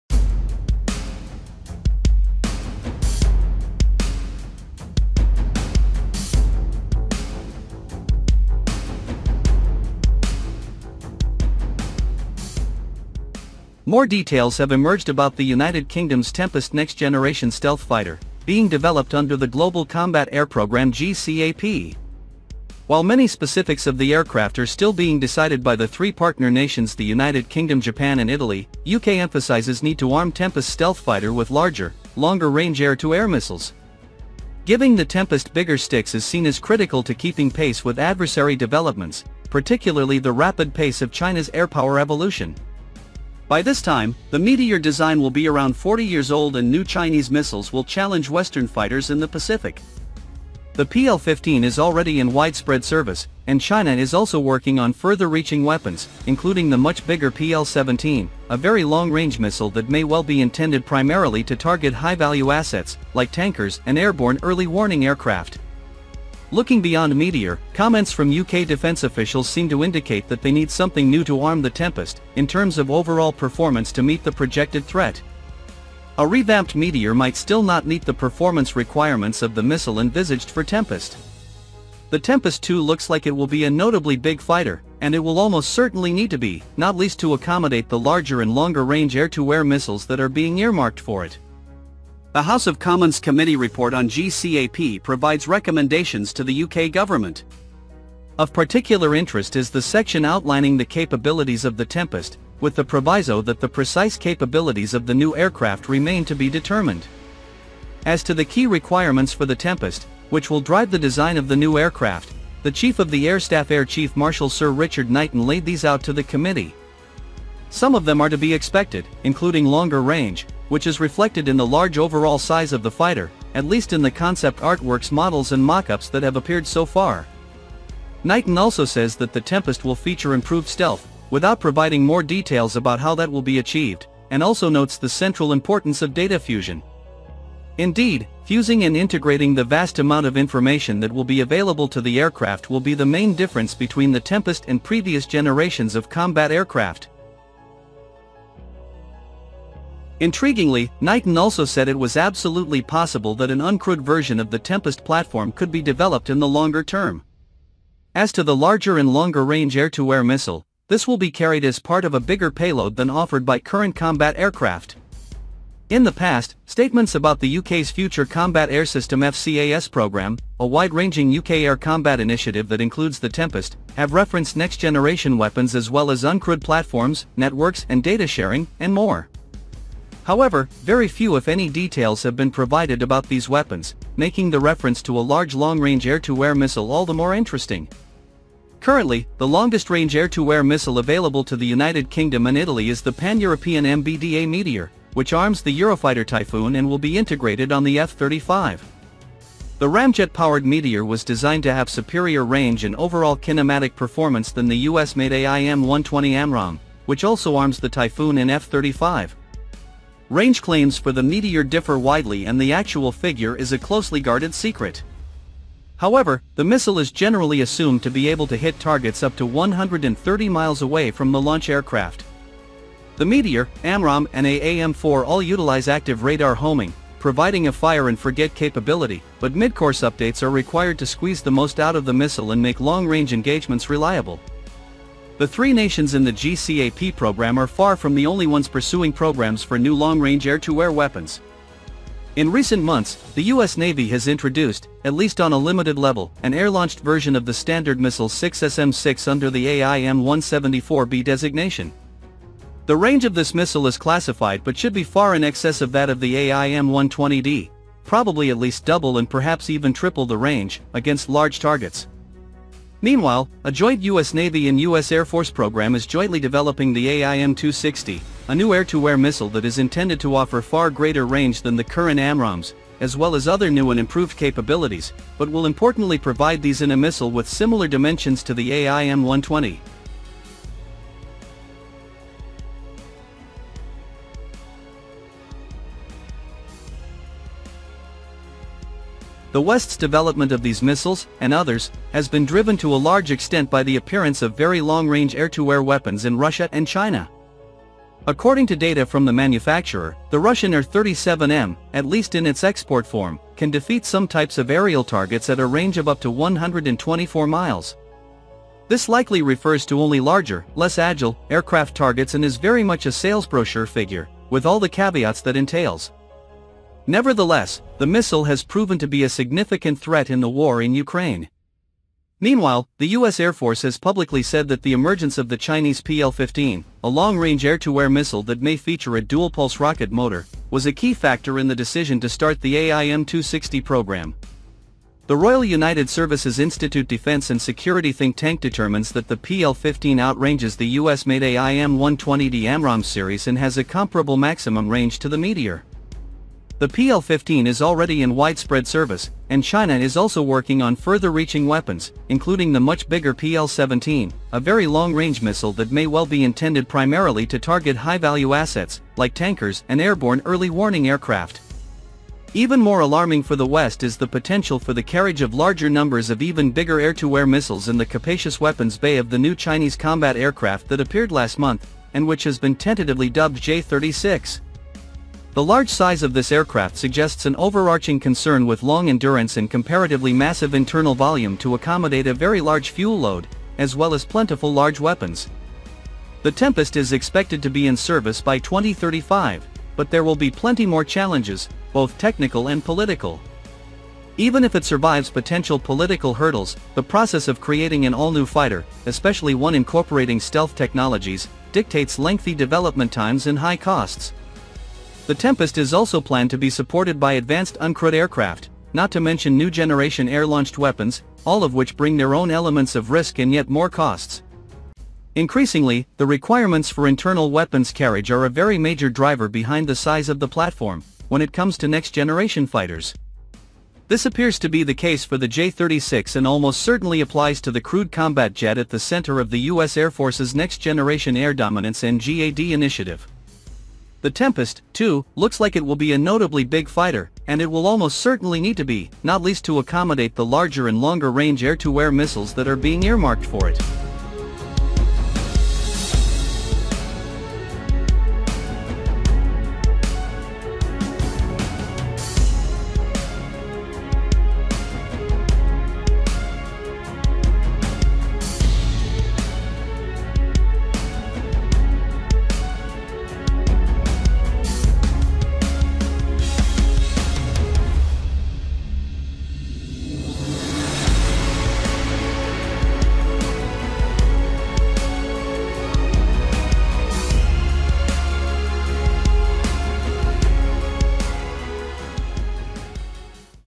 UK enlarges Tempest jet to carry Larger, Longer Range Missiles challenge to China's 6th-gen fighter
More details have emerged about the United Kingdom’s Tempest next-generation stealth fighter, UK Emphasizes Need To Arm Tempest Stealth Fighter With Larger, Longer Range Air To Air Missiles. The Tempest, too, looks like it will be a notably big fighter, and it will almost certainly need to be, not least to accommodate the larger and longer-range air-to-air missiles that are being earmarked for it.